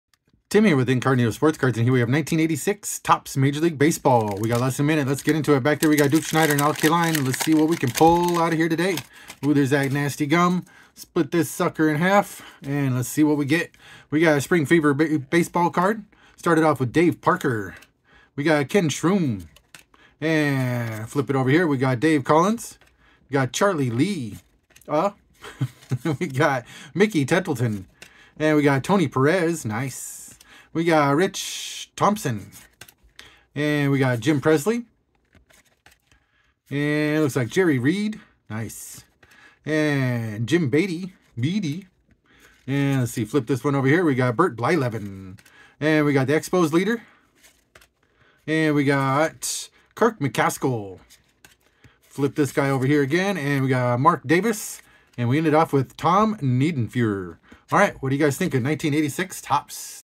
1986 Topps Baseball
We have another classic pack opening of 1986 Topps Major League Baseball Cards! I really like this set with the simple design really nice. What do you think of this set?